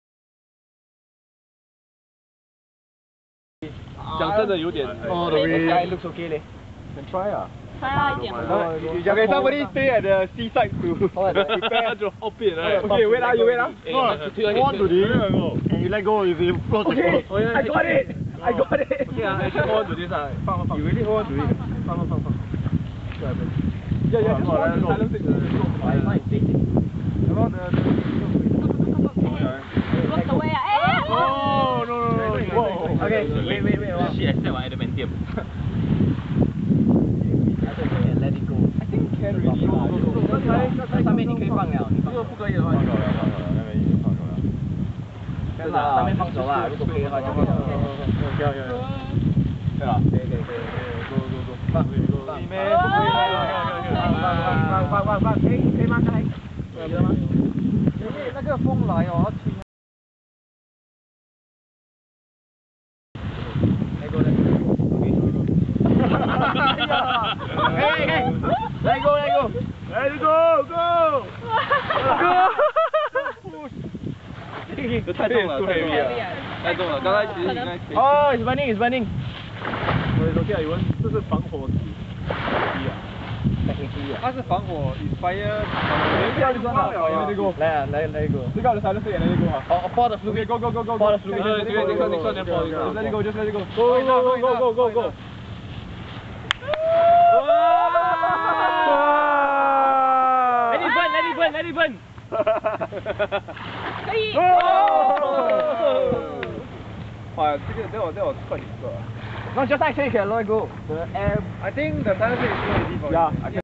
Another attempt with lightstick.wmv
Another attempt at lighting the lantern, this time Edric suggested we tie a lightstick at the bottom for more effect..